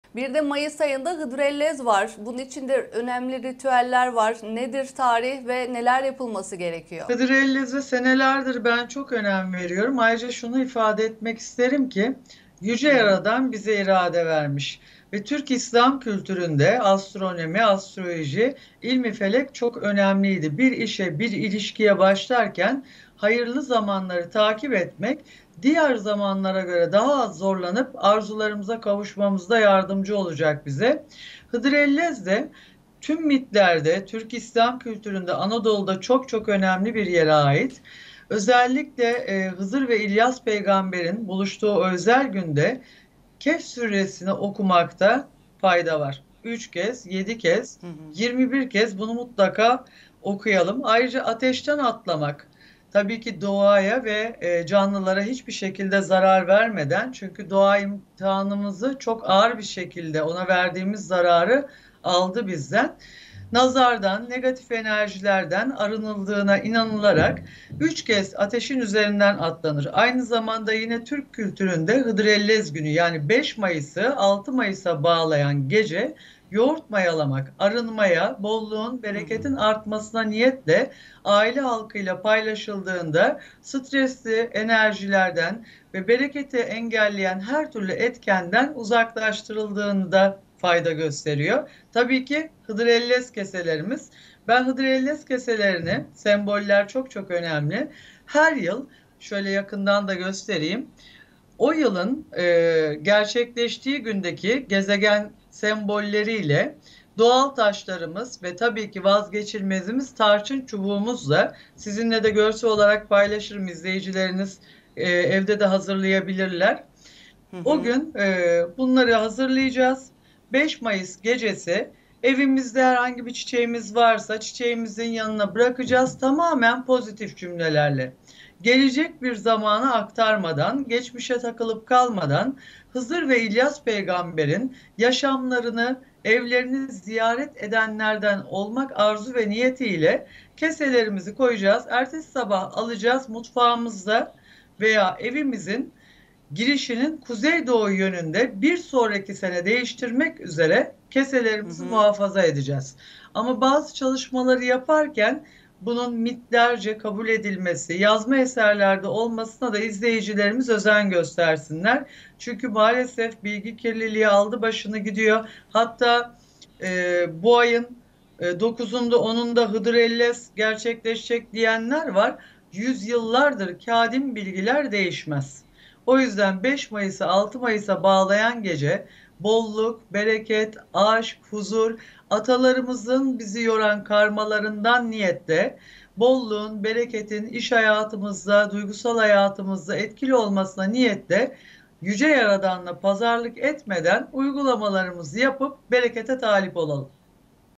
5 mayıs hıdrellez ritüelleri neler? Hıdırellez günü neler yapılır? Dr Astrolog Şenay Devi açıkladı
-Hıdırellez ne zaman, 5 Mayıs mi 6 Mayıs mı?
-Hıdırellez günü neler yapılır?
-Hıdırellez nasıl dilek dilenir?
-Hıdırellez gecesi neler yapılmalıdır?
-Hıdrellez ritüelleri neler?
Aleyna Horasan'ın sorularını Dr Astrolog Şenay Devi cevapladı.